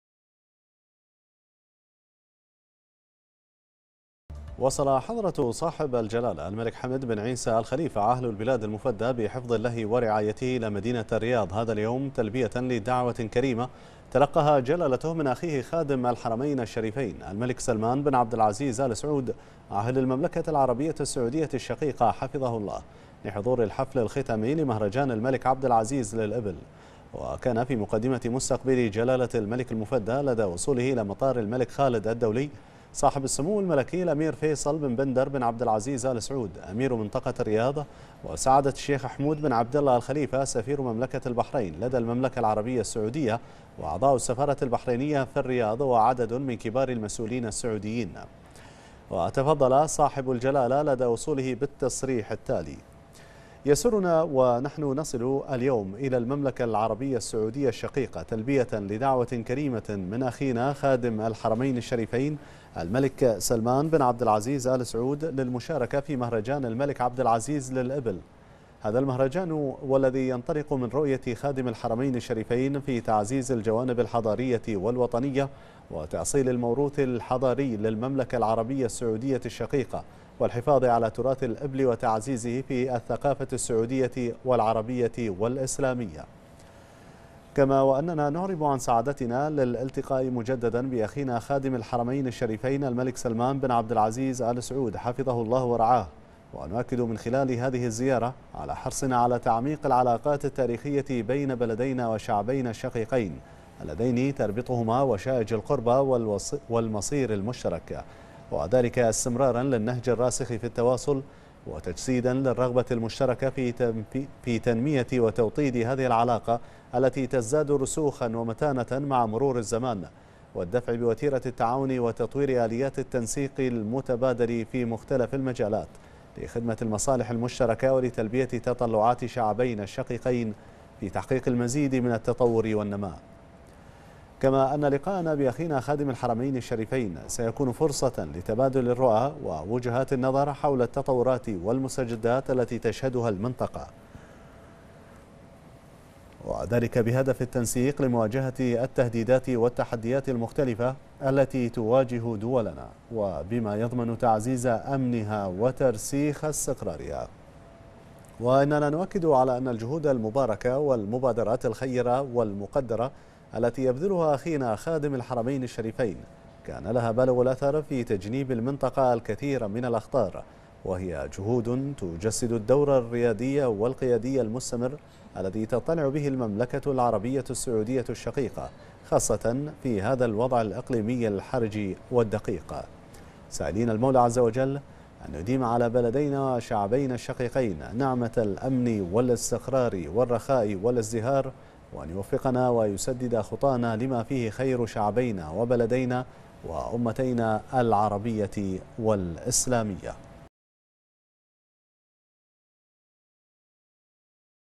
البحرين: جلالة الملك المفدى يصل إلى الرياض لحضور الحفل الختامي لمهرجان الملك عبد العزيز للإبل
الرياض في 01 فبراير/بنا/ وصل حضرة صاحب الجلالة الملك حمد بن عيسى آل خليفة عاهل البلاد المفدى بحفظ الله ورعايته إلى مدينة الرياض هذا اليوم تلبية لدعوة كريمة تلقاها جلالته من أخيه خادم الحرمين الشريفين الملك سلمان بن عبد العزيز آل سعود عاهل المملكة العربية السعودية الشقيقة حفظه الله لحضور الحفل الختامي لمهرجان الملك عبد العزيز للإبل.